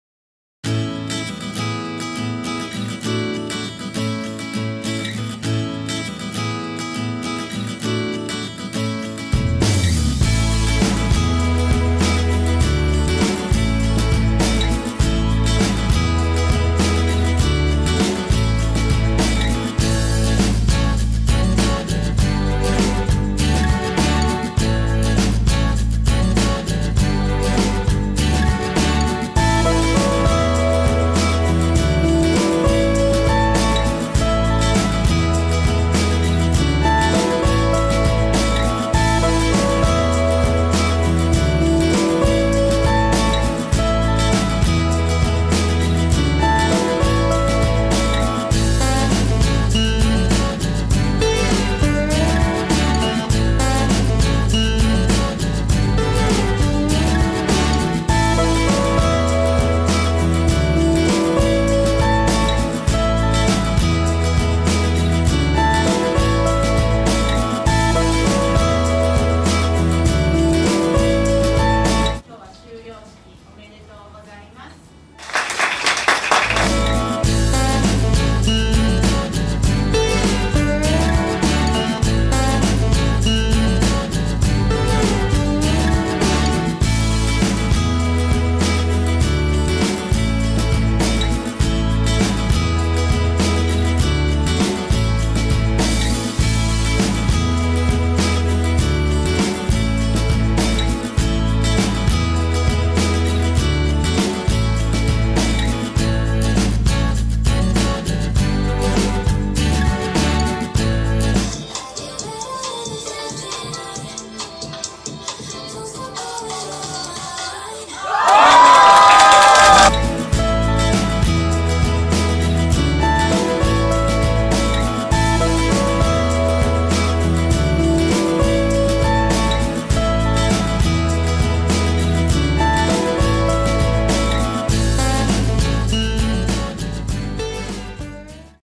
Memorable Moments! - Highlights of Fall 2023 Kyoto Consortium for Japanese Studies (KCJS)
Watch how KCJS students fully enjoyed their study abroad experience in Kyoto!
- Social meeting with local college students
- Okayama Trip
- Yuzen Dyeing Workshop
- Sutra Copying Experience
- Host families
- Fun activities in class
... and KCJS Awards & Talent Show!